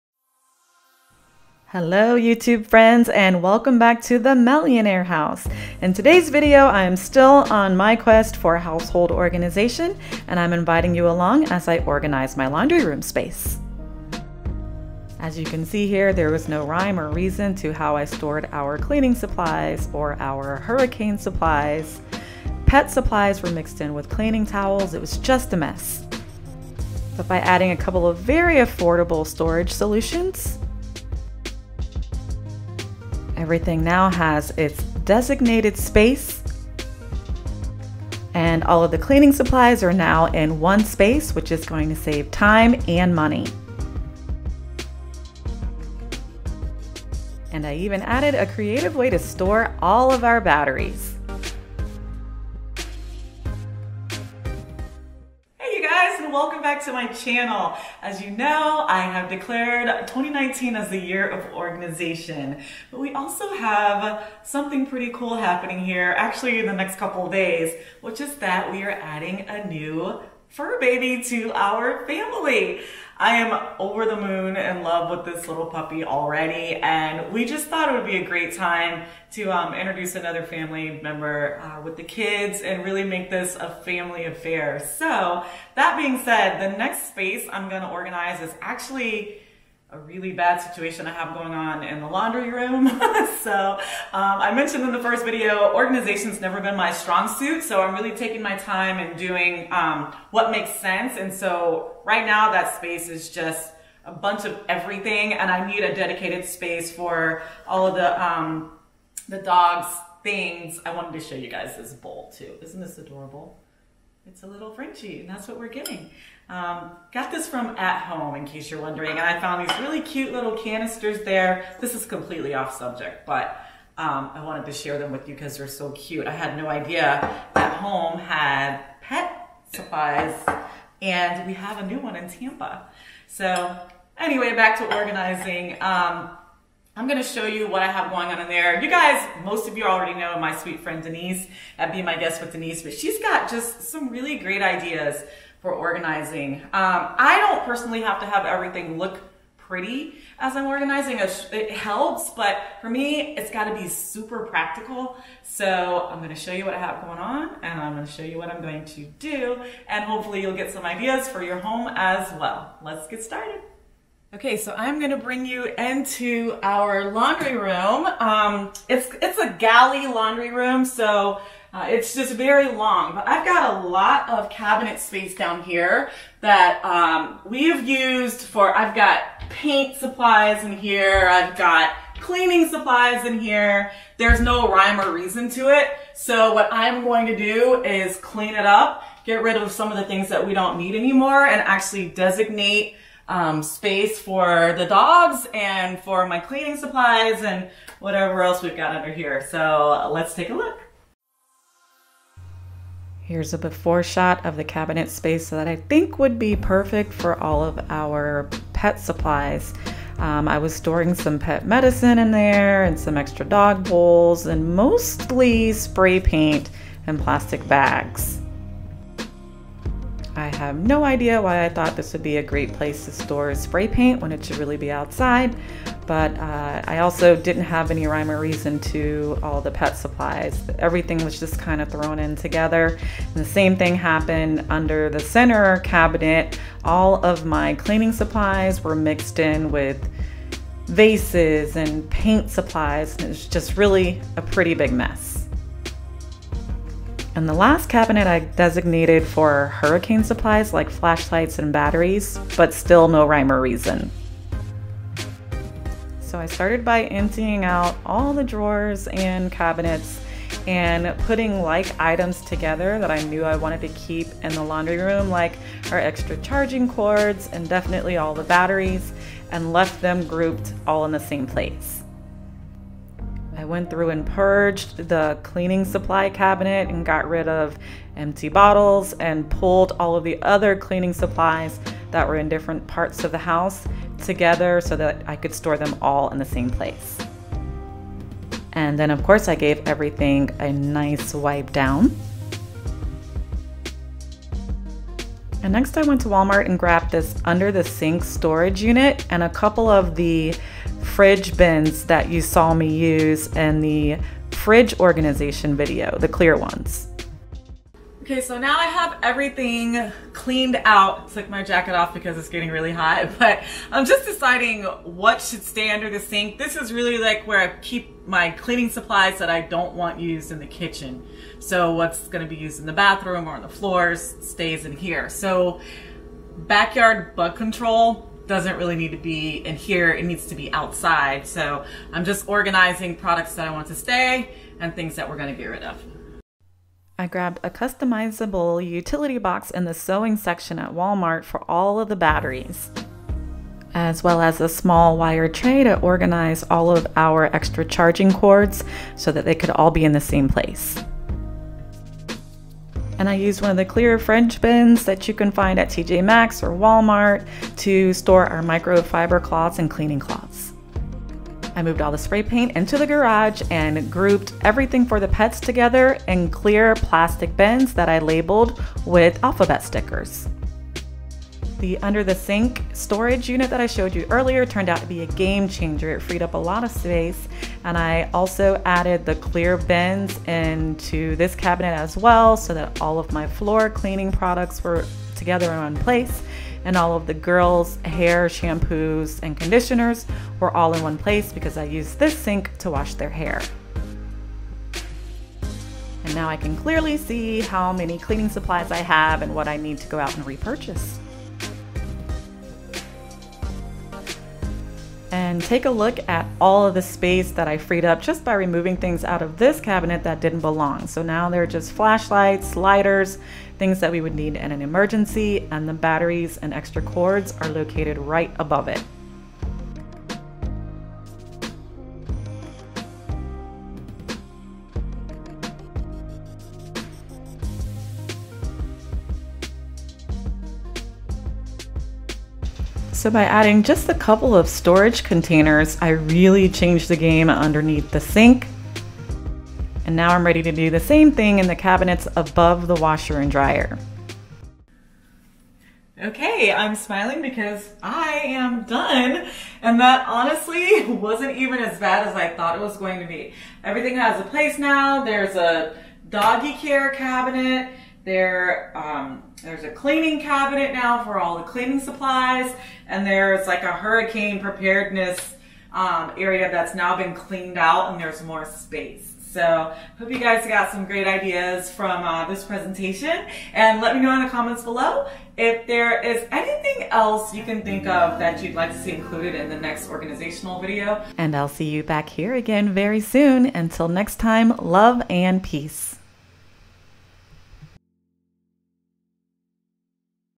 ORGANIZE WITH ME | Laundry Room Organization Ideas
Today I’m sharing how I organized my laundry room using a couple of products from Walmart and TJ Maxx.  I hope you get some ideas that will help you in your homes as well.